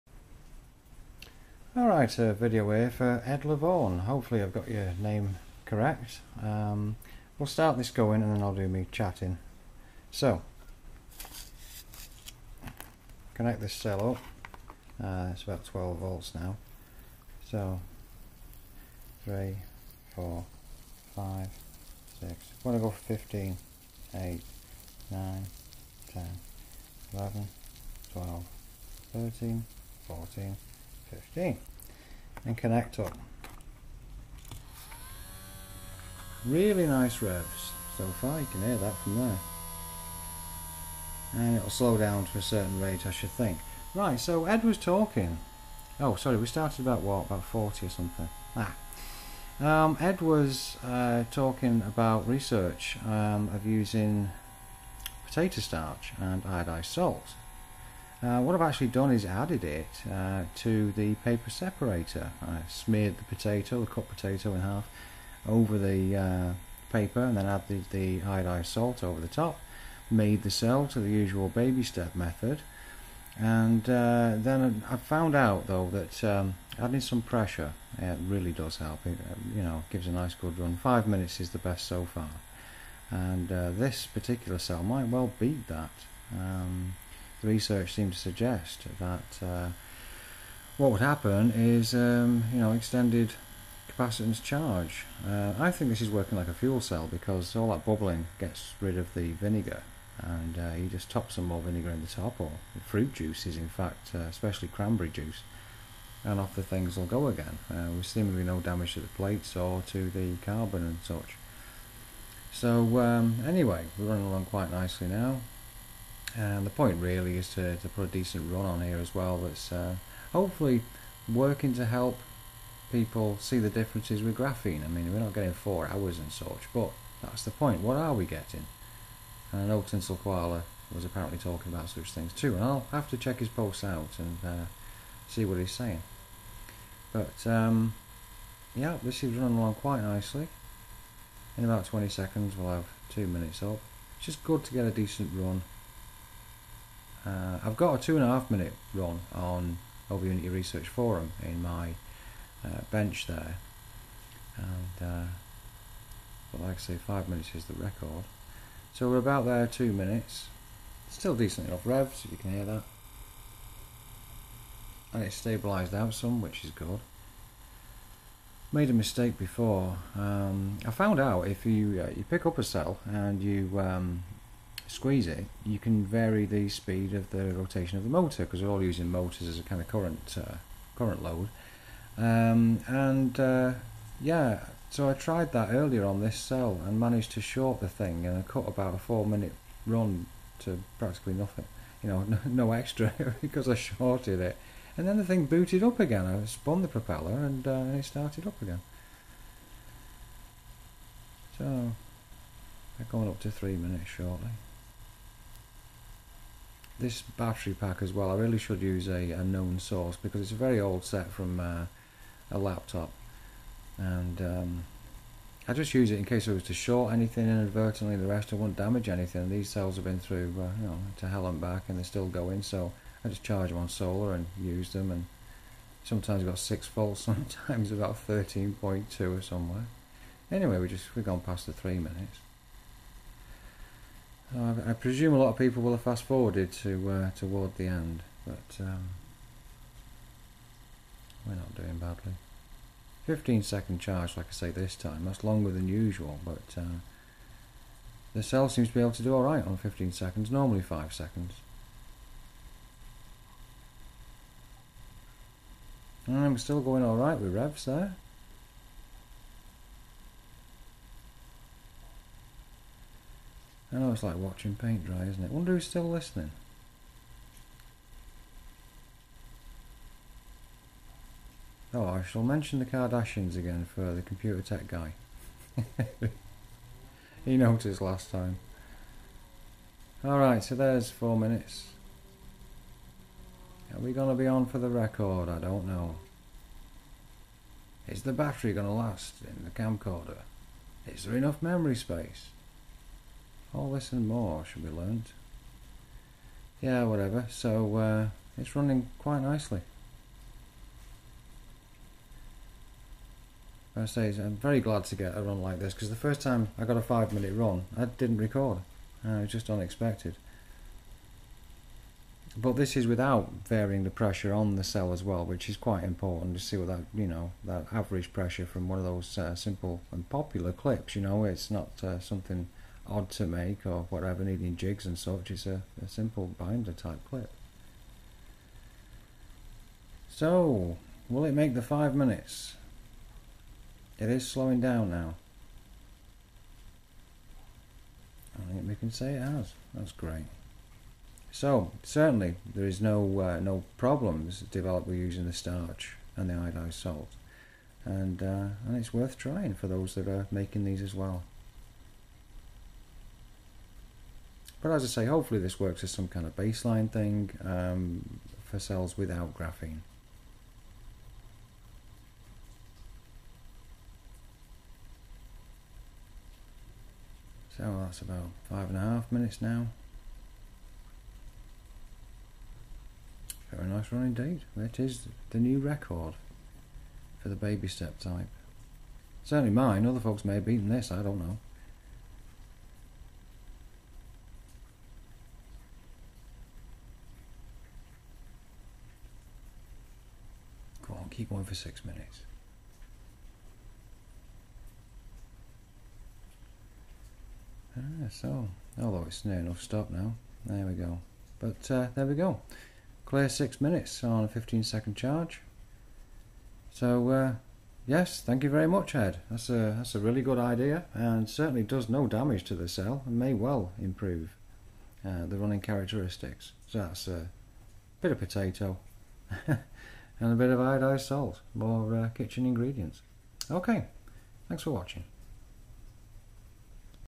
6 minutes supercap motor run - no graphene
Ed Lovvorn suggested that research he had read about may be beneficial. It would seem to do no harm at all !

The cell is a regular 'baby step' type (see video on channel for how to make one).
Charge was 12V for 15 seconds.